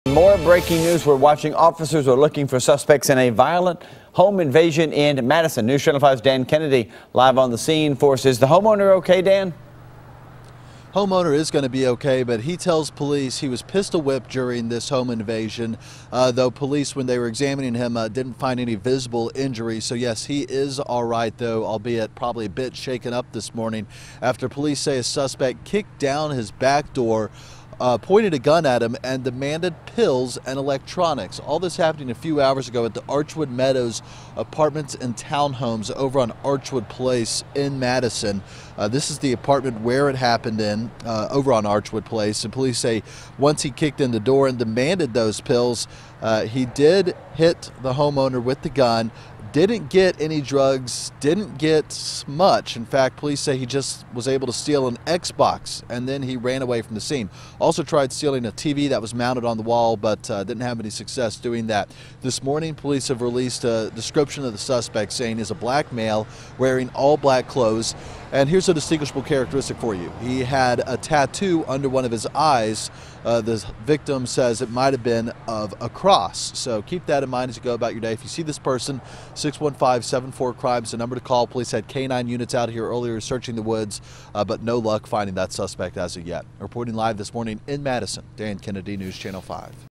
Suspect Sought In Madison Home Invasion
A search has gotten underway in Madison for a suspect wanted in connection with a violent home invasion in Madison.